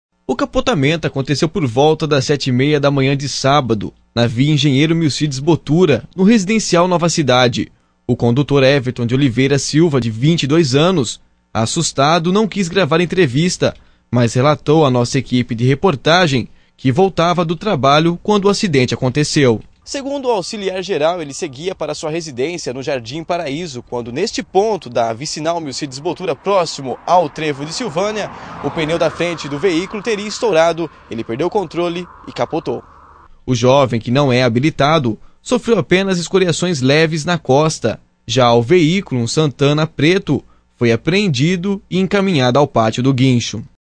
Jovem capota carro na Via Milcíades Bottura
O capotamento aconteceu por volta das 7h30 da manhã deste sábado (8) na Via Eng. Milcíades Bottura, no Residencial Nova Cidade em Matão (SP). Segundo informações do condutor do carro, um Volkswagen Santana preto, ele seguia para sua casa no Jardim Paraíso sentido centro/bairro, quando próximo ao trevo de Silvânia, o pneu dianteiro direito do carro teria estourado, fazendo com que o mesmo perdesse o controle do veículo.

O Auxiliar Geral, de 22 anos, não é habilitado e sofreu apenas escoriações leves na costa. O veículo foi apreendido e recolhido ao pátio.

Mais informações e todos os detalhes,você confere nos telejornais da TVM (canal 58 UHF) desta segunda-feira (10).